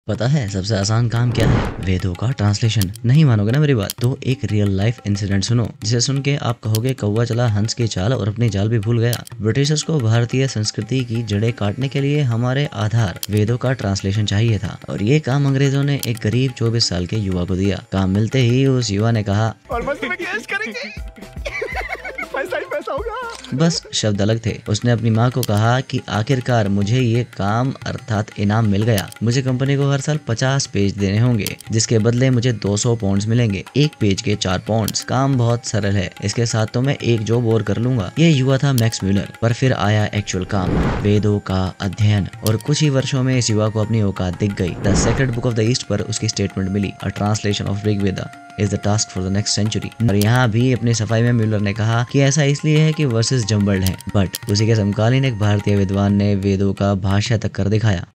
max muller and vedas | hypocrisy
max muller an incompetent person given a job out of his league. How? let's examine in one minute.

I don't own some of the audio and pictures used in this video.

footages and images and music :
PIXABAY.COM
Google.com